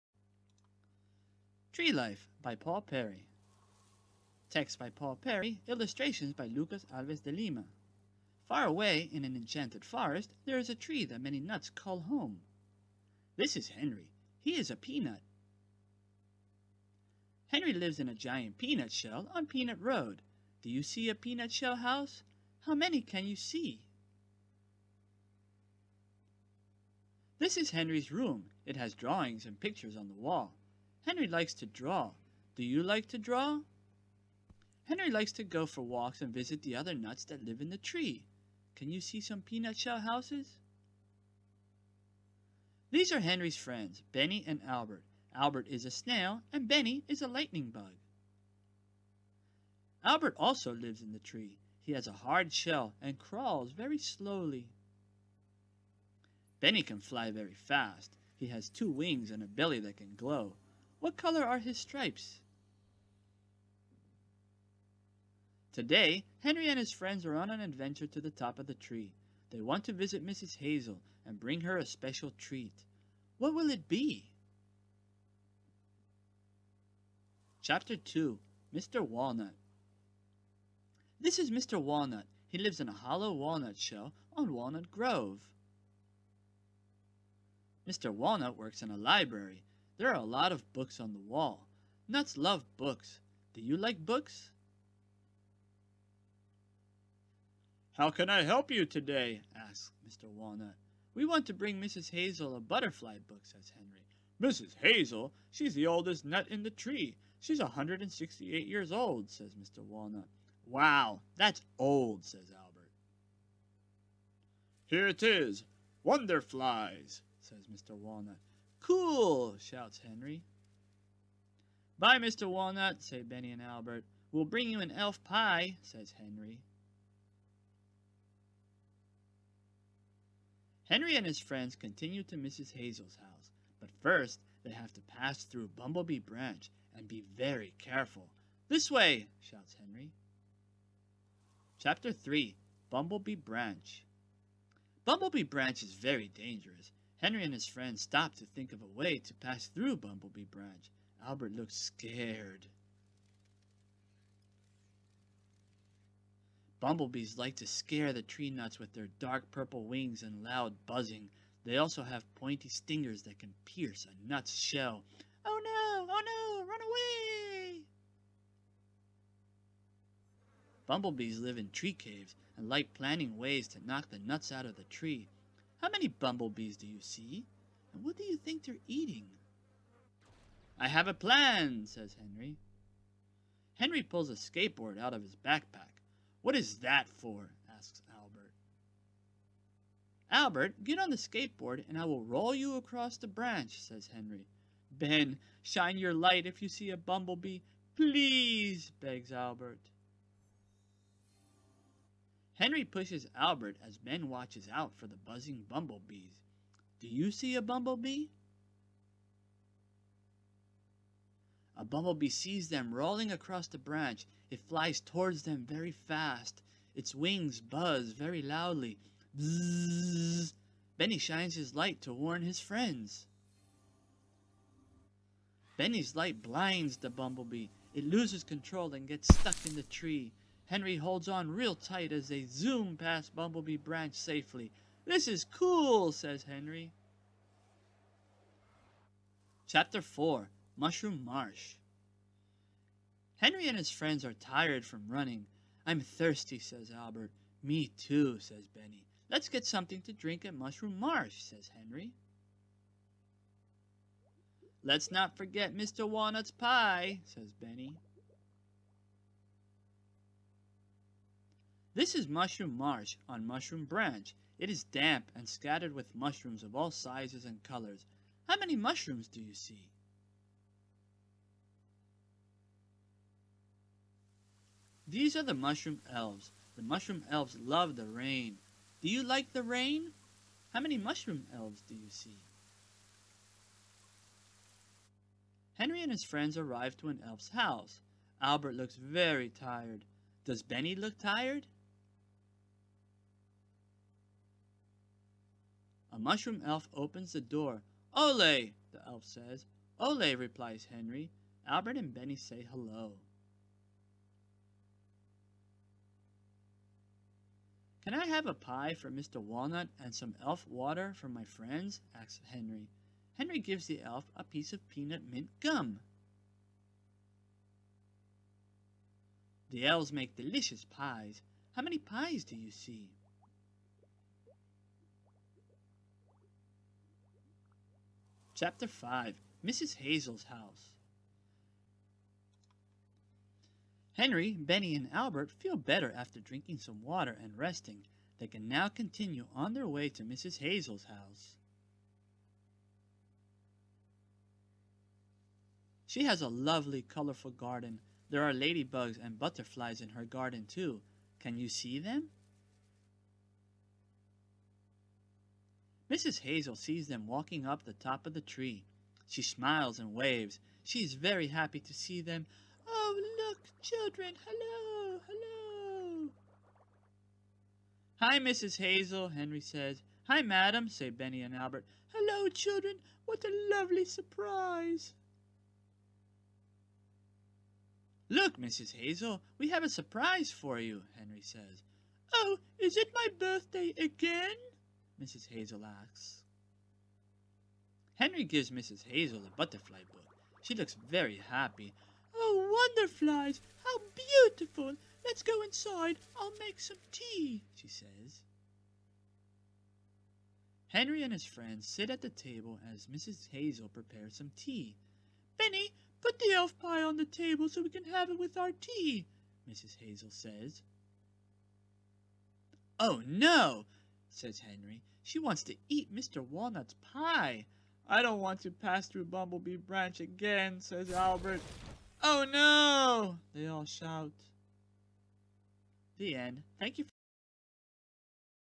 Tree Life by Paul Perry
U.S. / Argentine Hybrid Author reads, "TREE LIFE" (2010)

Written in 2001, during the worst economic crisis in Argentine history, Paul Perry creates this gem of a book; inspired by his newborn son, Henry, a bumblebee, and a snail that lived in his yard.

Dedicated to the innocence of children.